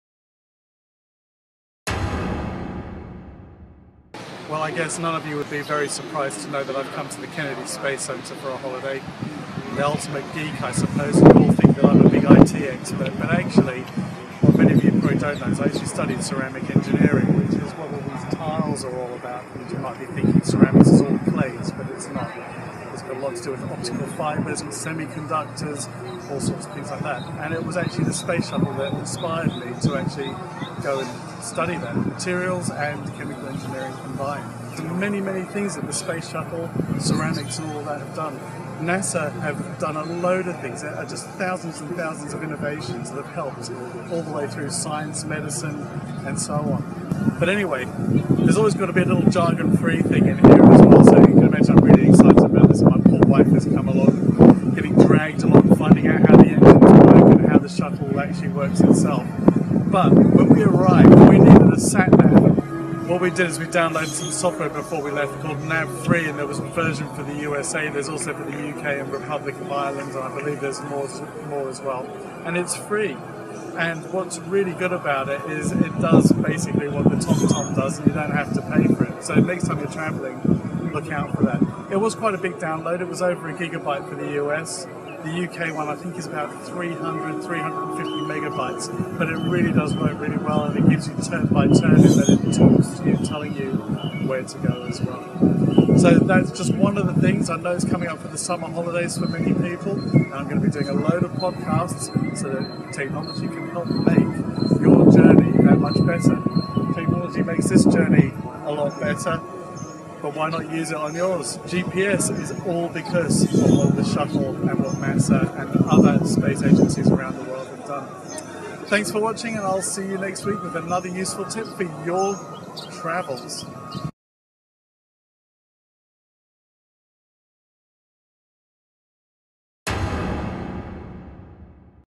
Free Sat Nav Software with Navfree
Why pay for a Tom Tom or other sat nav if you have an iPhone or iPad. Navfree is free sat nav software for the iPhone and iPad. for more podcasts.